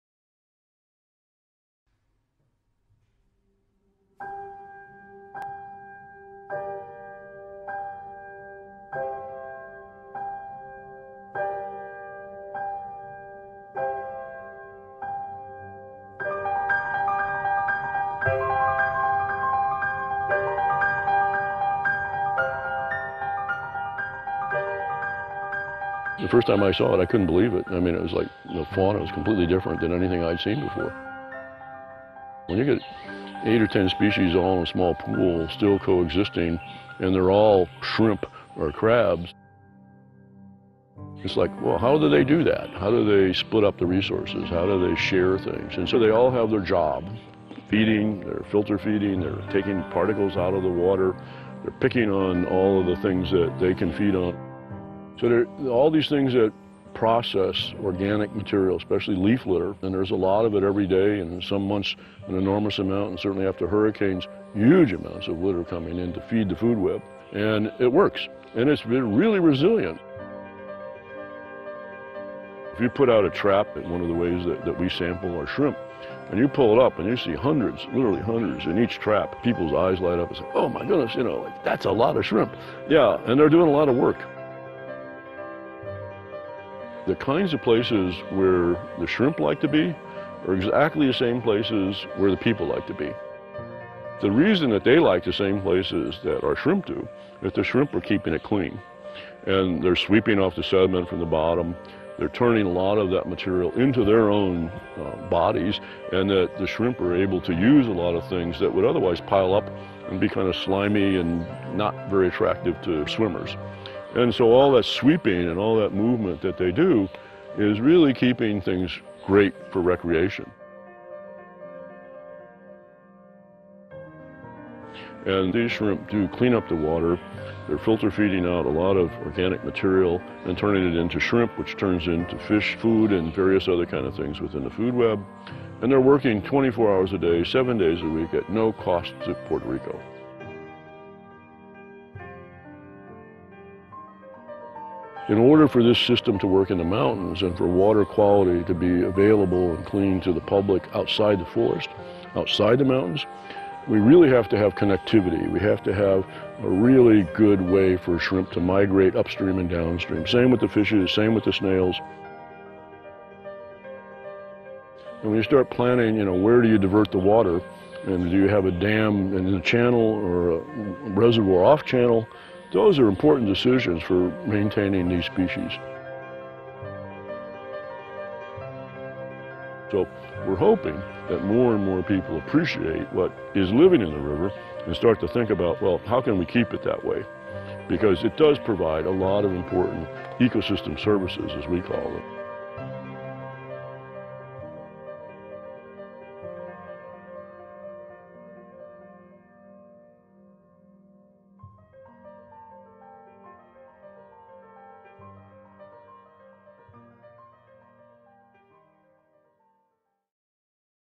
ShrimpInc HD
Freshwaters Illustrated video about shrimp ecology in El Yunque